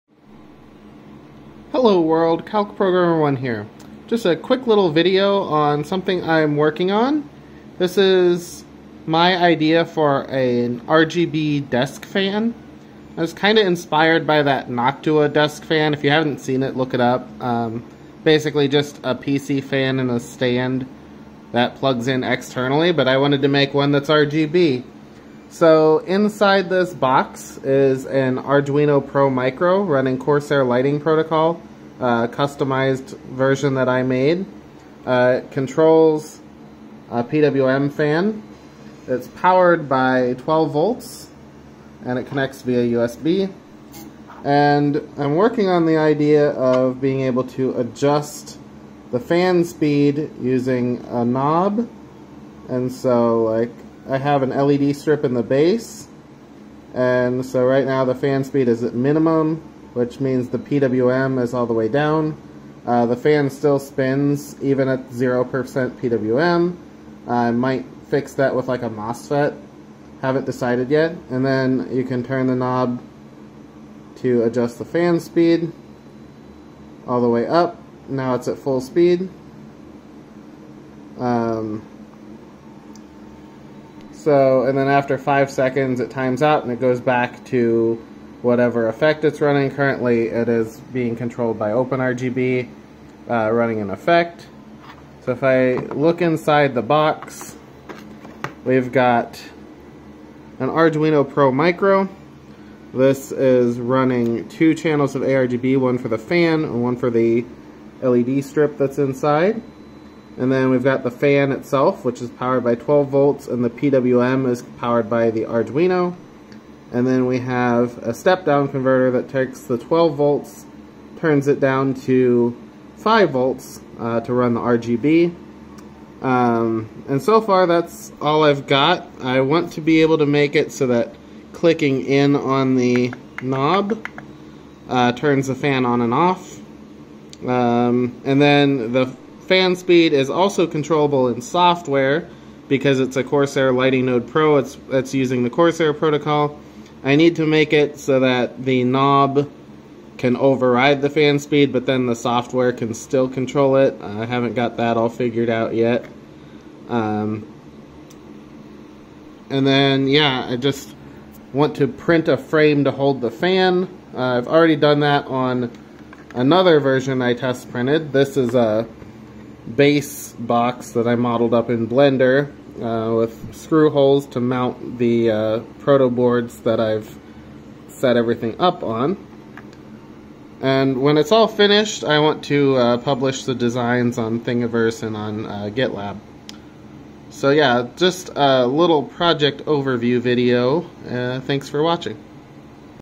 OpenRGB Desk Fan - Proof of concept and control test
After seeing Noctua's PC fan "desk fan" I want to do the same but with RGB.  I had some spare ARGB fans and am working on a design for a 3D printed housing and Arduino Pro Micro based controller to control both the lighting as well as the fan speed.  I want to add a knob to adjust the fan speed but also allow the speed to be controlled in software.